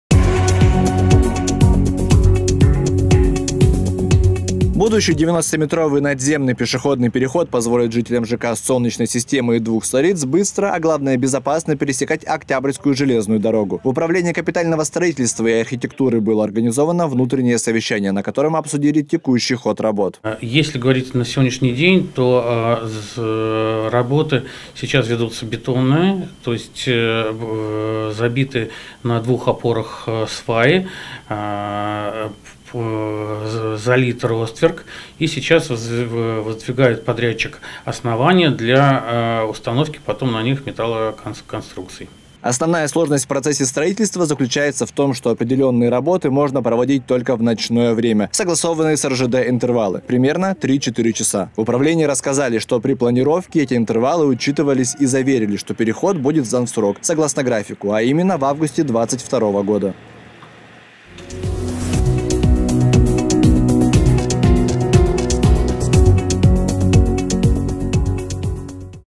В Химках прошло совещание по строительству надземного перехода. 17.12.2021
Будущий 90-метровый надземный пешеходный переход позволит жителям ЖК «Солнечной системы» и «Двух Столиц» быстро, а главное – безопасно пересекать Октябрьскую железную дорогу. В Управлении капитального строительства и архитектуры было организовано внутреннее совещание, на котором обсудили текущий ход работ.